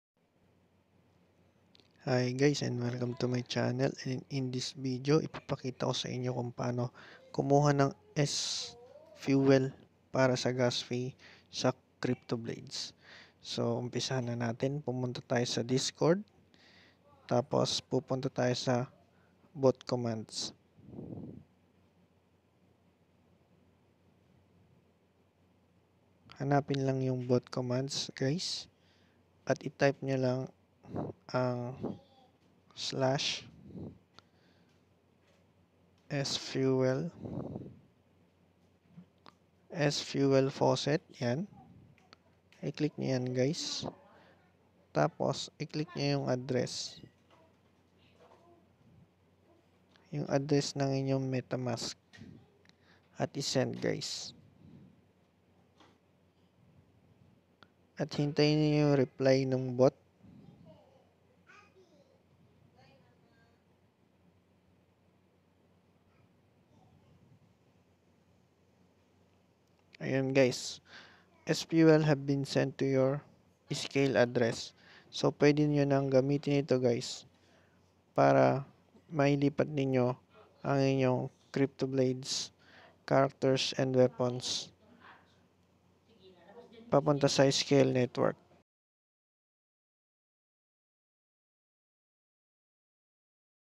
SFUEL GAS FEE FOR SKALE NETWORK FREE ON DISCORD
punta s bot commands para kumuha ng SFUEL

/sfuelfaucet adress

Sharing my experience on how to claim SFUEL free on discord

POLYGON TO SKALE
BNB TO SKALE
OEC TO SKALE
HT TO SKALE

HOW TO BRIDGE CRYPTOBLADES OKEX